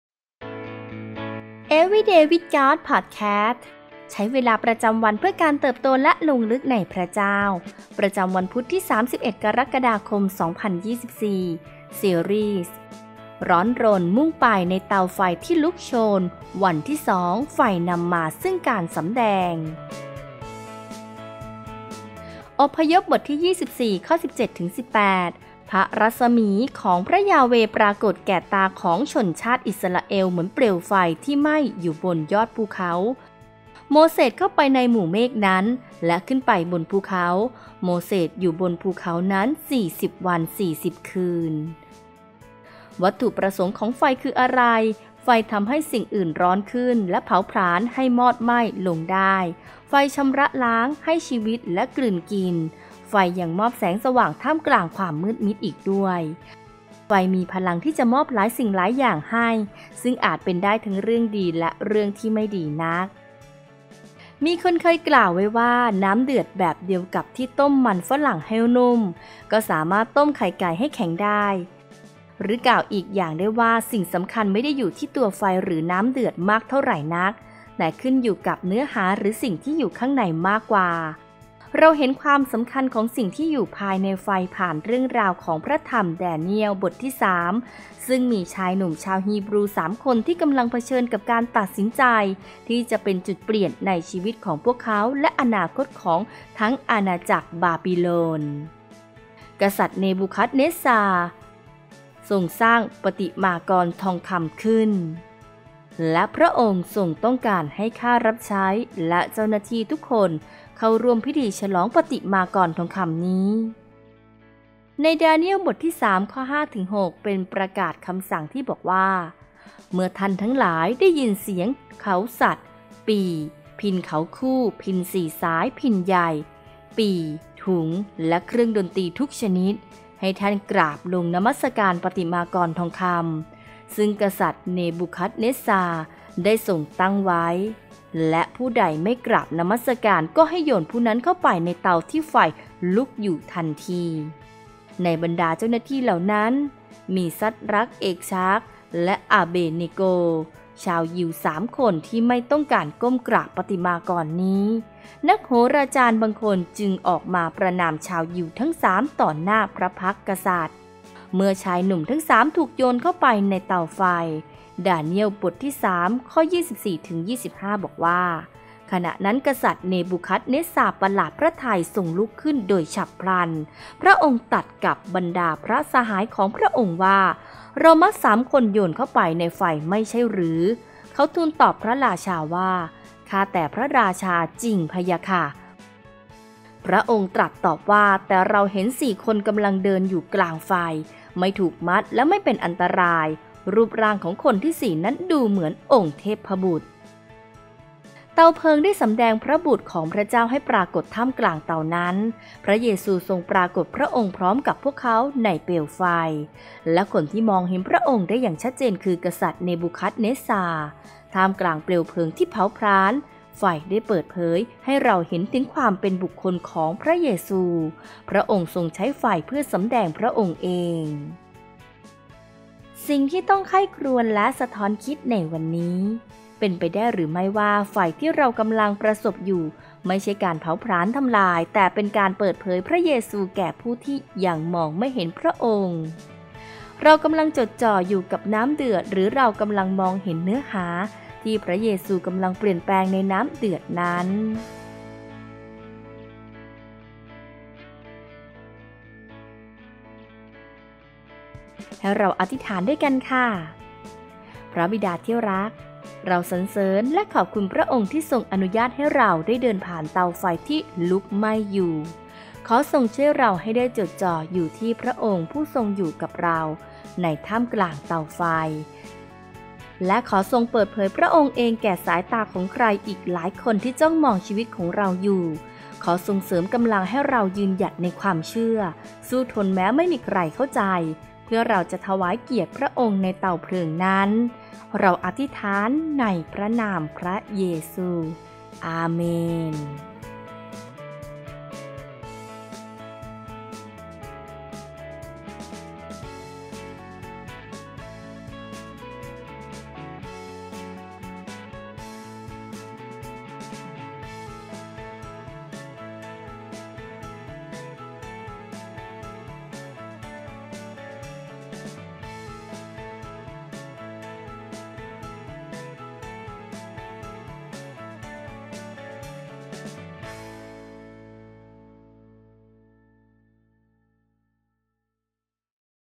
31/7/2024 Every Day with God | ซีรีส์ : ร้อนรน มุ่งไป ในเตาไฟที่ลุกโชน - The Fire | วันที่ 2/3
ประจำวันพุธที่ 31 กรกฎาคม 2024

ซีรีส์ : ร้อนรน มุ่งไป ในเตาไฟที่ลุกโชน
วันที่ 2 ไฟนำมาซึ่งการสำแดง

OVERVIEW :
เป็นไปได้ไหมที่พระเจ้าจะทรงอนุญาตให้เราก้าวผ่าน “เตาไฟที่ลุกอยู่” ในชีวิตเพื่อเราจะสามารถตอบคำถามยากที่ท้าทายความเชื่อของเรา และรู้ว่าอะไรคือสิ่งที่เราเชื่ออย่างแท้จริง?  ความท้าทายเหล่านี้เป็นสิ่งจำเป็น และเป็นวิธีการของพระเจ้าที่เปิดโอกาสให้เราได้เติบโต รู้จัก และมั่นใจในความเชื่อของเรา  นี่คือความจริงอันทรงพลัง 3 ประการ เมื่อเราต้องก้าวผ่านเตาไฟที่ลุกอยู่

บทใช้เวลากับพระเจ้านี้แปลและเรียบเรียงจากแผนการอ่าน The Fire โดยพันธกิจ Juan Garcia Enterprises